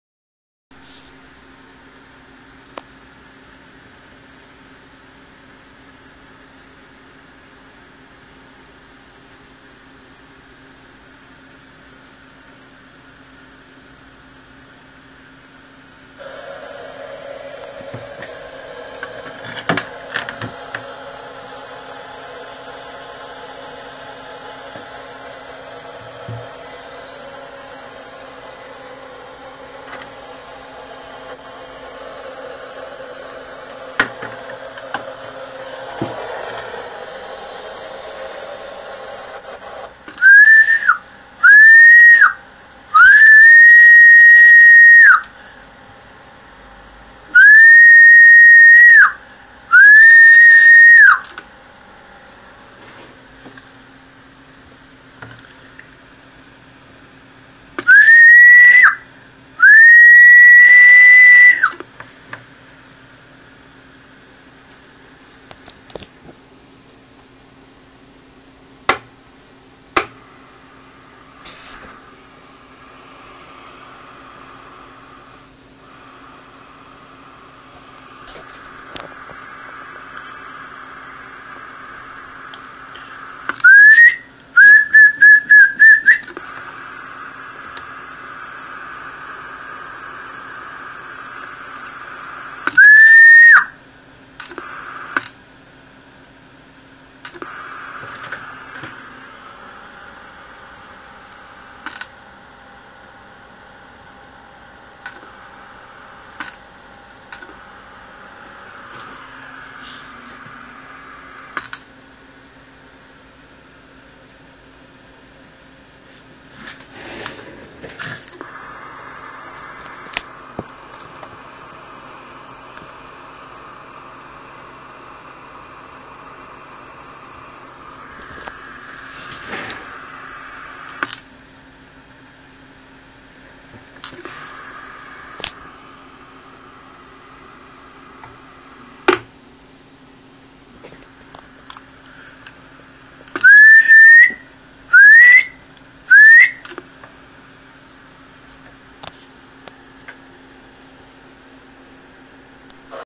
testeando Ranger AR-3500 , no funciona.
Pruebas realizadas para comprobar estado de un Ranger AR-3500 versión de 120w , el equipamento usado fue una carga ficticia MFJ-264 , medidor Diamond SX-600 y fuente de 30 amperios , como se puede observar el equipo no saca la potencia que debería sacar , consumo maximo de 7 amp y potencia máxima unos 35 w aprox.